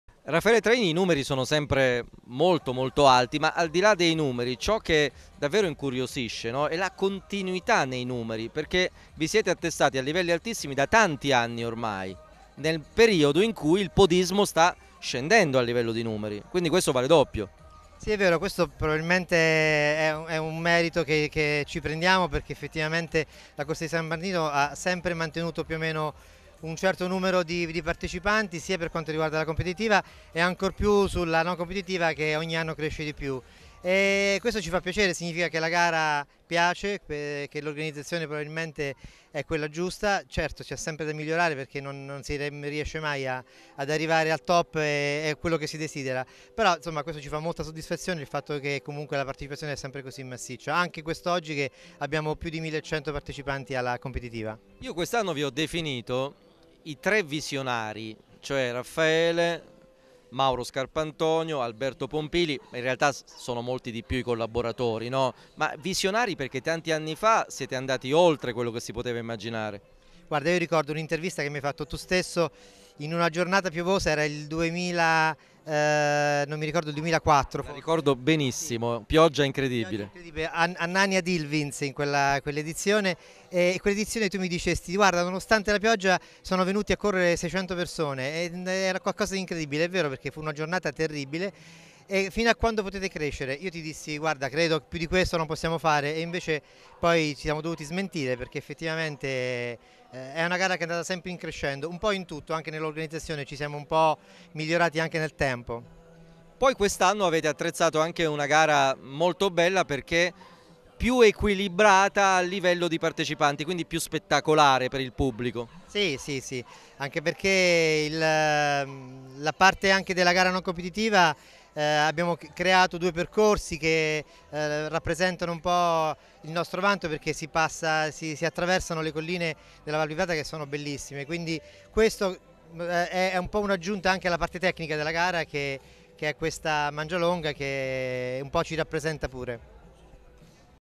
Corsa di San Martino, grande successo per la 23 edizione
Il keniano Julius Rono tra gli uomini e la burundese Cavaline Nahimana tra le donne si sono aggiudicati l’edizione 2019 della Corsa di San Martino di Controguerra, in provincia di Teramo. Anche quest’anno numeri importanti per la gara podistica giunta alla 23esima edizione e che ha contato su 3400 iscritti di cui 1100 alla prova competitiva di 15 km